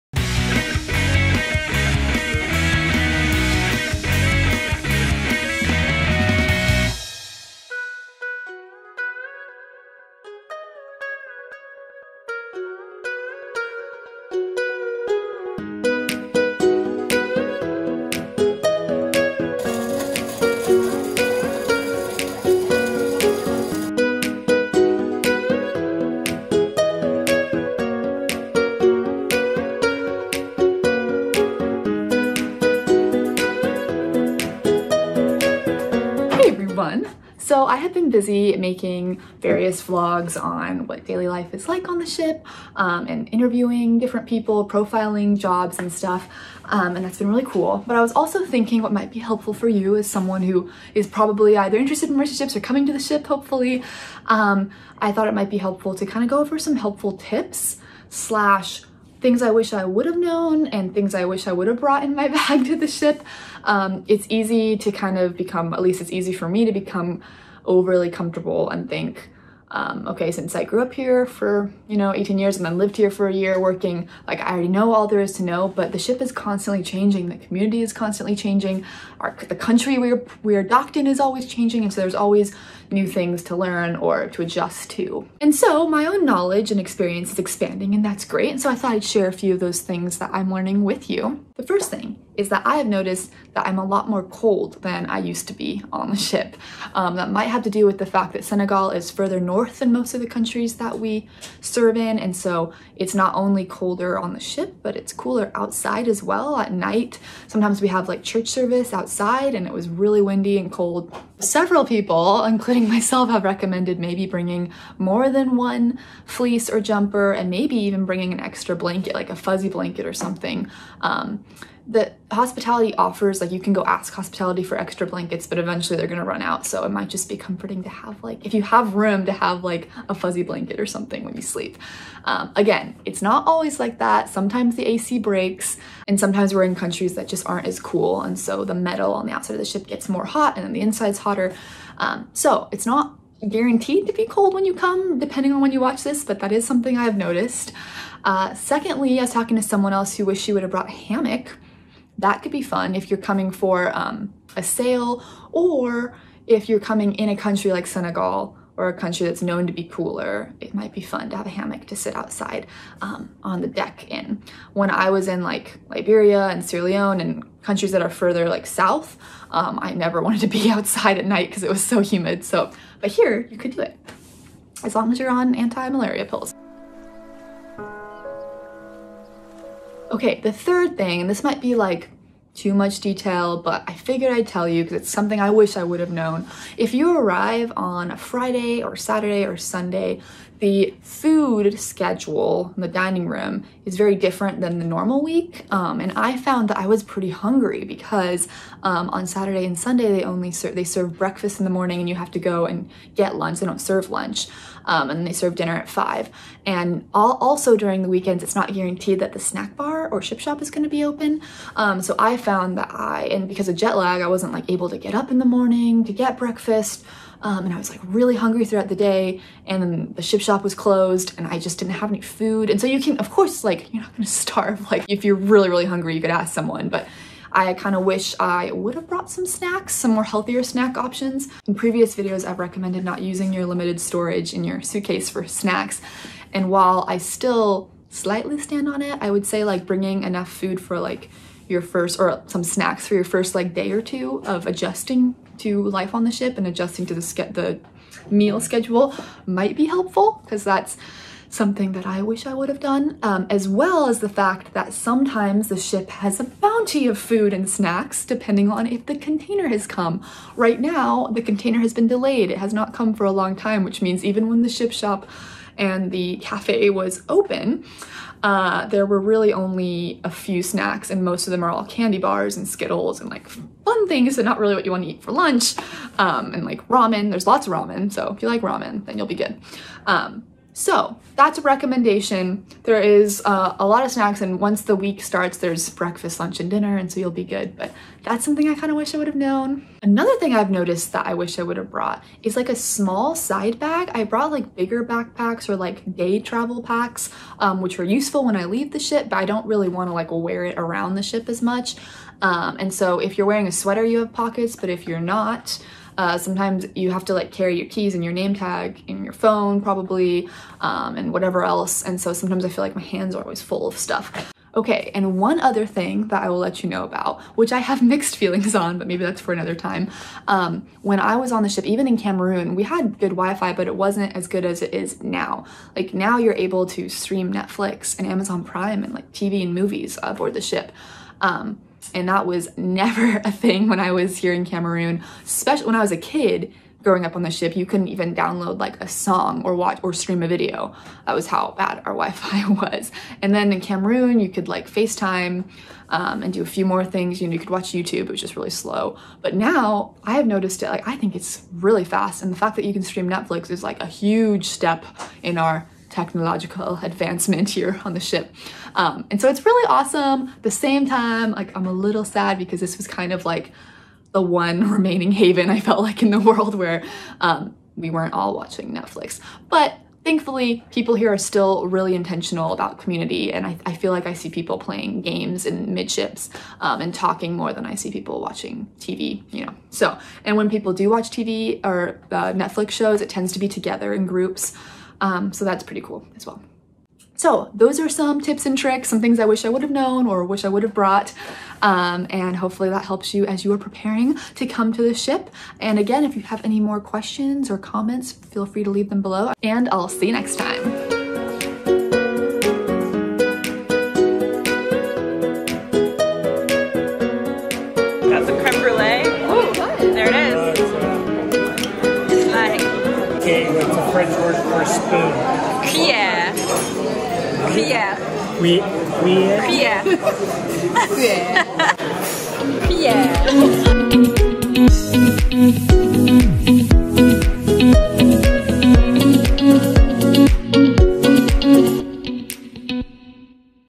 My Mercy Ship Adventure #77 -- Things I Wish I Would have Known BEFORE I Got to the Ship
Ship life is very familiar to me but no matter how long you've been with the ship, things are always changing in Mercy Ships culture and life and so these are just a few tips and tricks I wish I would have known before I actually got to the ship!